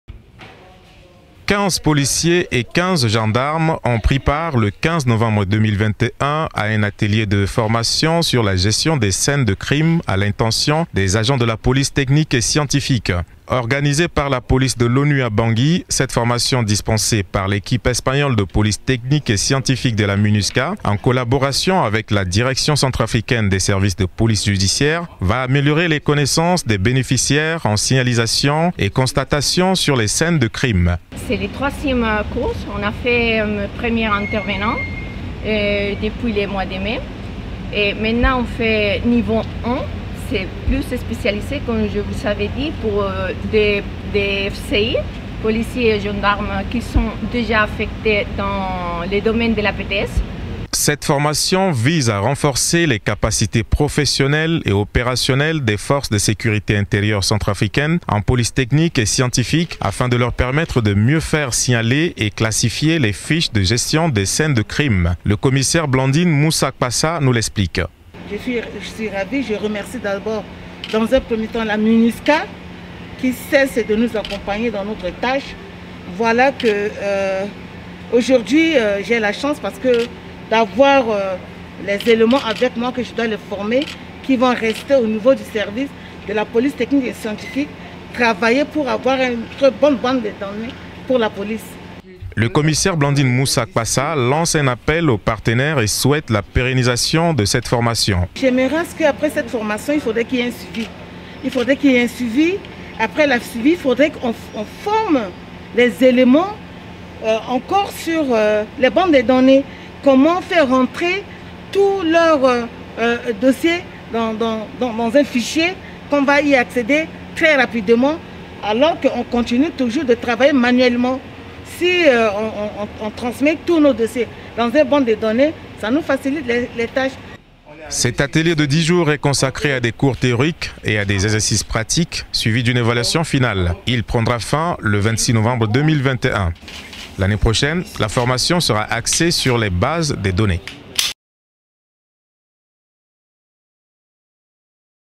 Renforcement des capacités des Forces de sécurité intérieure sur la gestion des scènes de crime
15 policiers et 15 gendarmes ont pris part, le 15 novembre 2021, à un atelier de formation sur la gestion des scènes de crime à l’intention agents de la police technique et scientifique.
Organisé par la Police de l’ONU, à Bangui, cette formation dispensée par l’équipe espagnole de police technique et scientifique de la MINUSCA en collaboration avec la Direction centrafricaine des services de police judiciaire, va améliorer les connaissances des bénéficiaires, en Signalisation et Constatation, sur les scènes de crime.